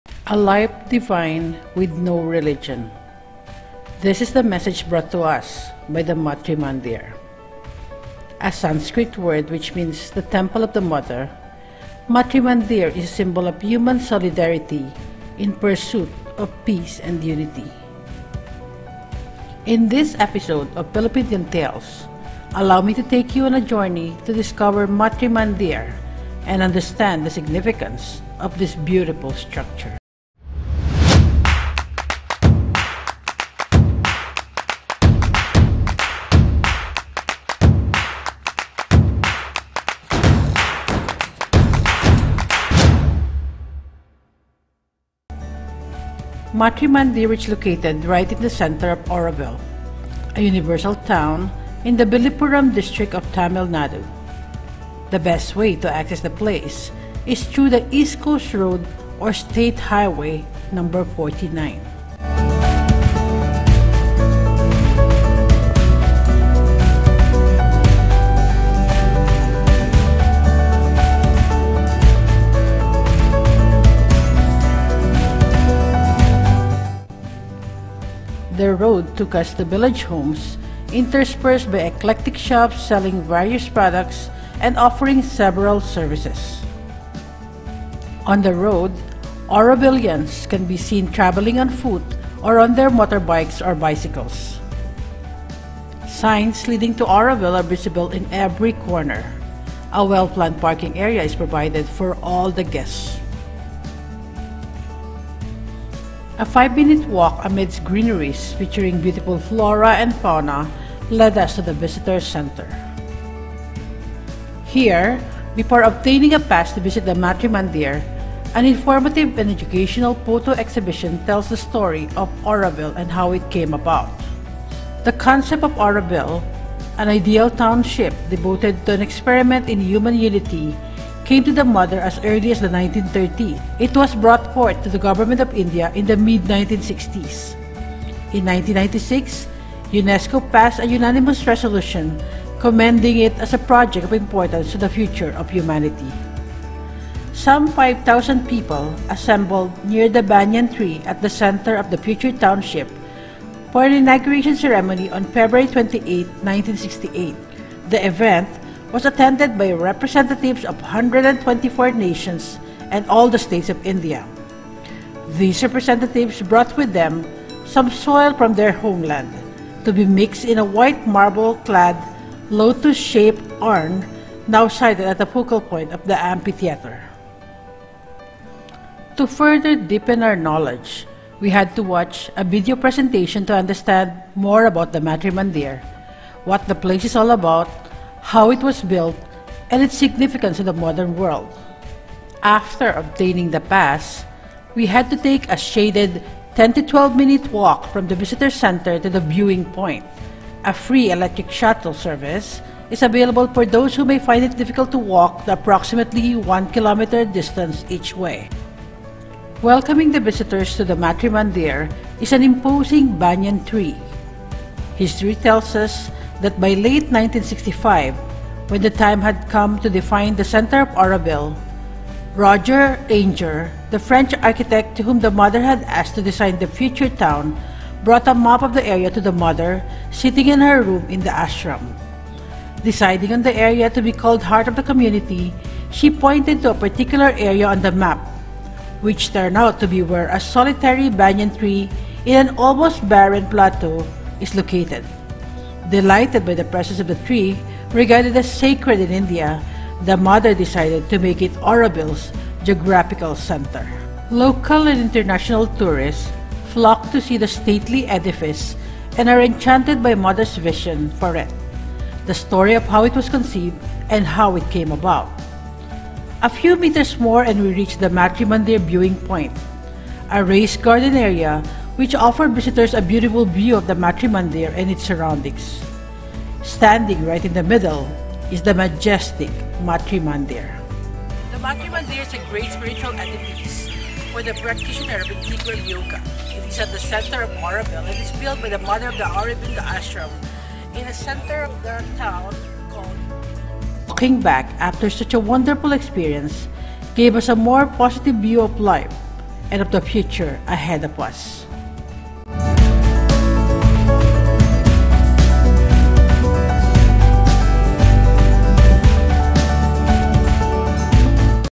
Matrimandir at Auroville - Filipindian Tales Vlog#7
The Matrimandir is an edifice of spiritual significance for practitioners of Integral yoga, in the centre of Auroville established by The Mother of the Sri Aurobindo Ashram. It is called soul of the city and is situated in a large open space called Peace. Filipindian Tales takes you on this peaceful journey.